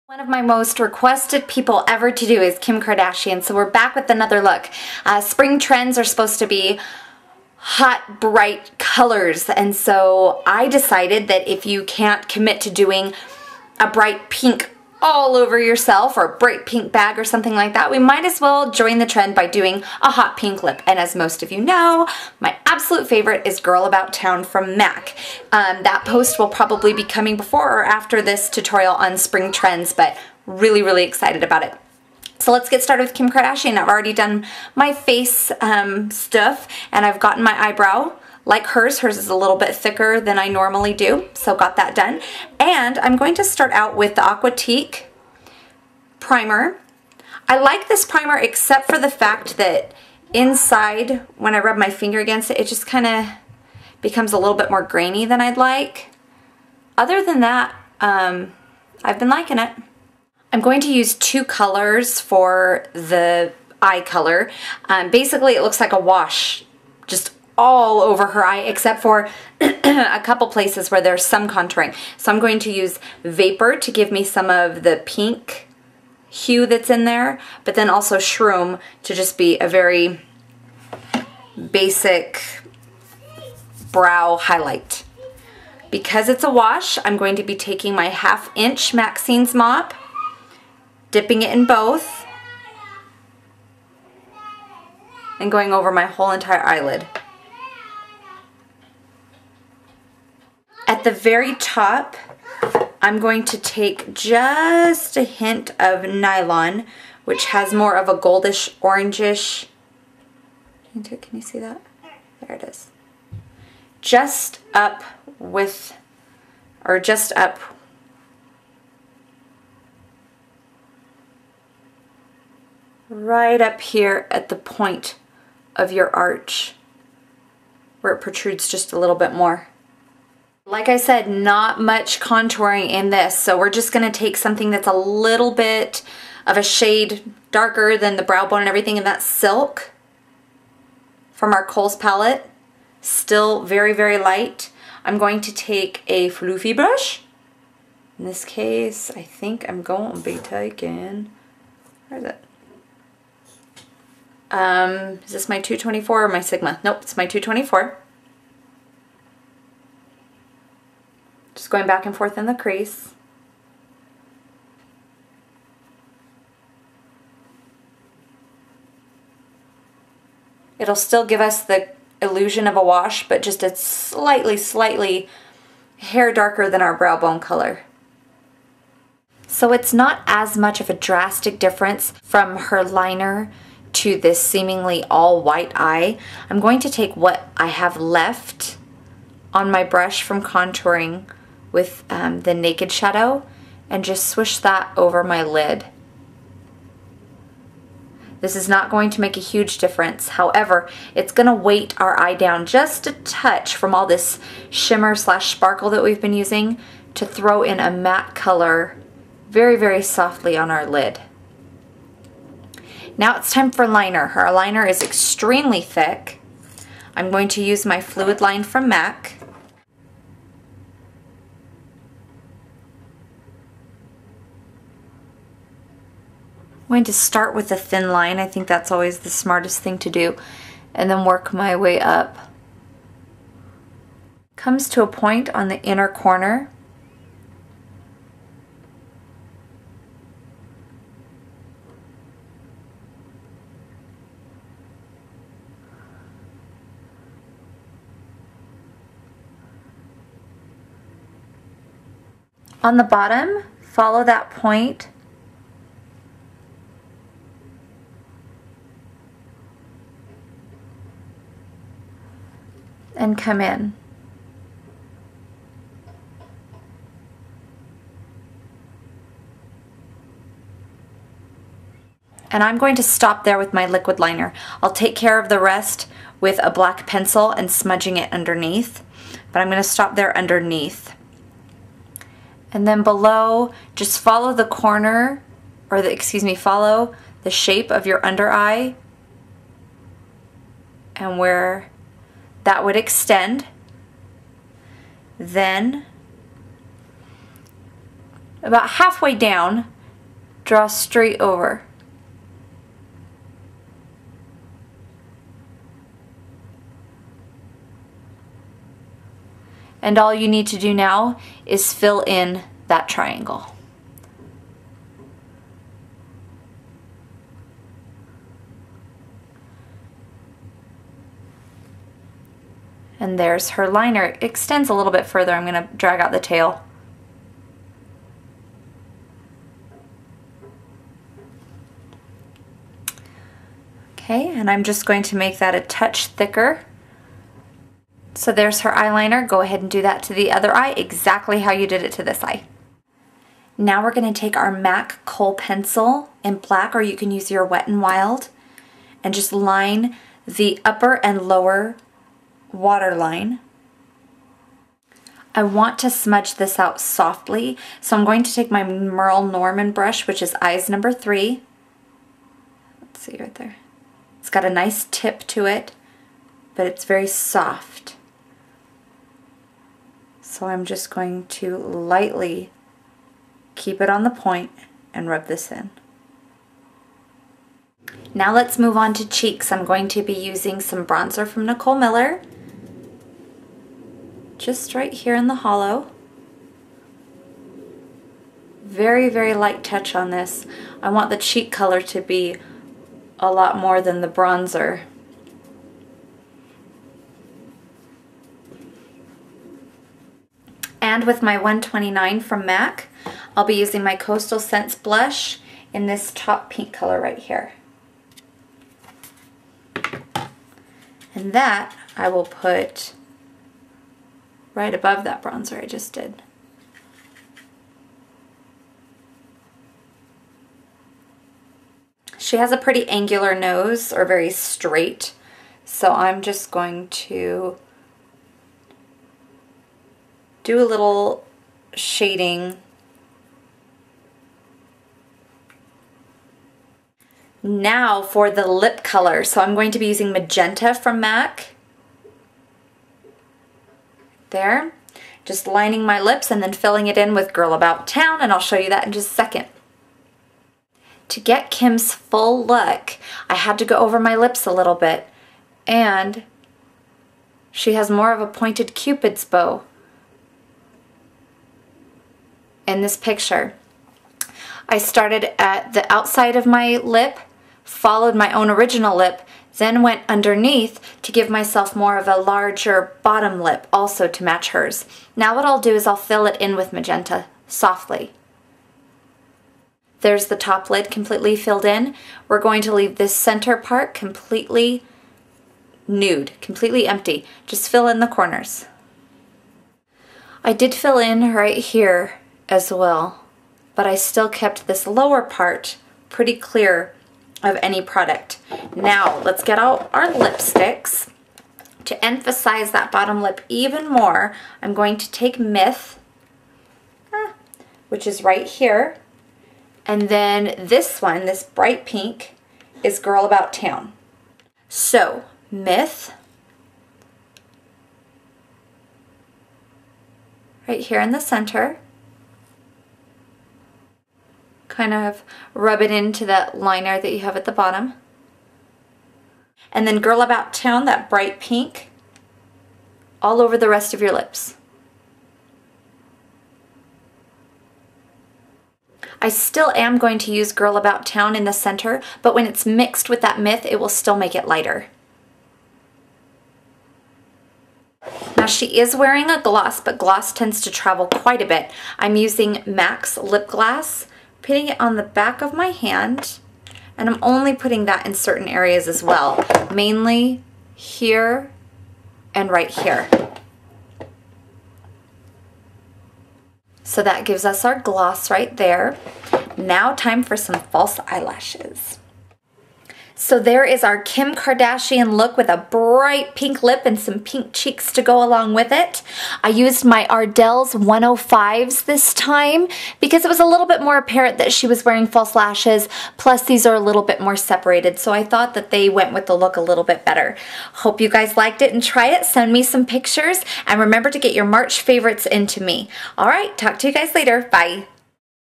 Kim Kardashian Tutorial
Spring trends for 2011 show an explosion of color! If you can't commit to a bright pink all over then try this look. A wash all over the eye with bold liner and a pop of bright pink lips. SO on trend =). To see the inspiration picture go to classycosmetics.blogspot.com.